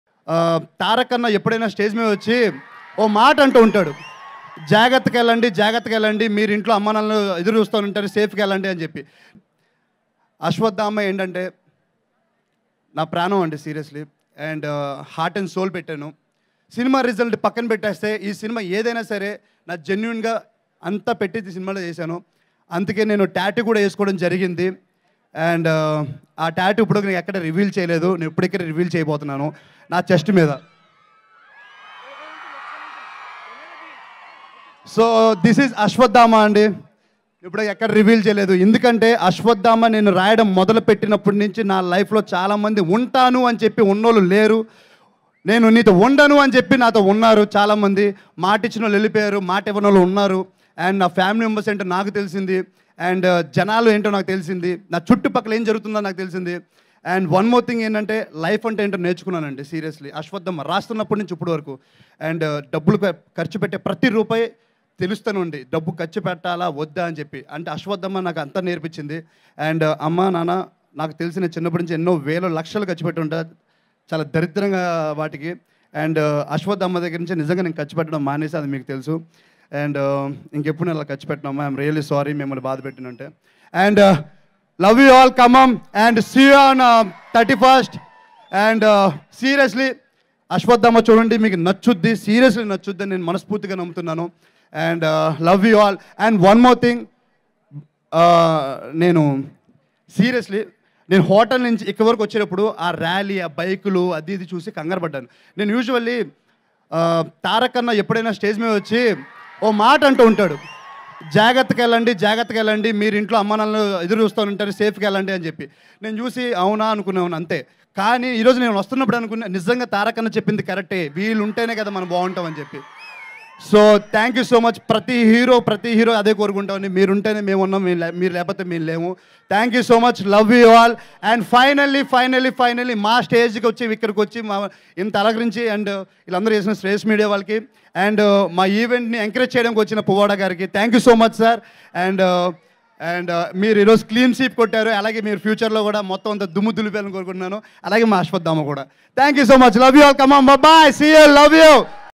Naga Shourya on Junior NTR comment at Ashwathama Movie Audio Launch | Mehreen Pirzada | Filmylooks
Watch Naga Shourya on Jr NTR comment at Ashwathama Movie Audio Launch | Mehreen Pirzada | Filmylooks